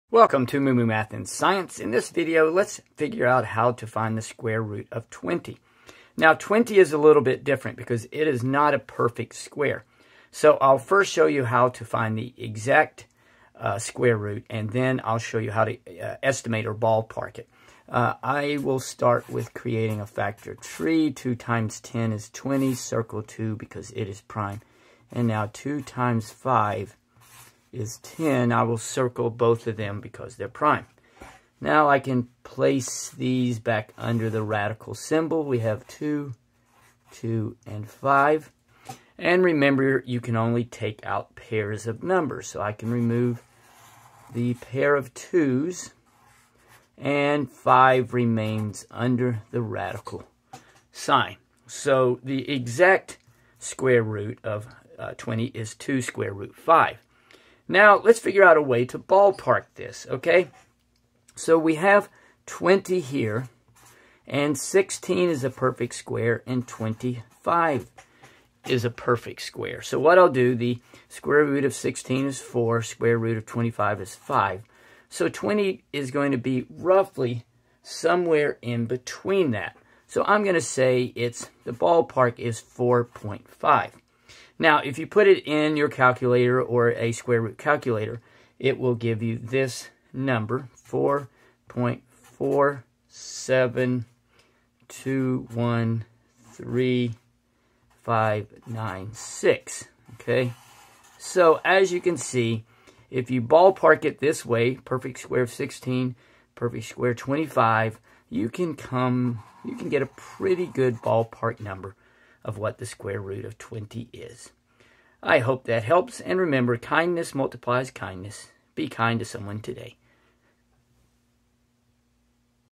Find the Square Root of 20
Learn how to find the square root of 20. The number 20 is not a perfect square. I cover how to find the exact square root and then a simple technique to find an estimate of the square root of twenty.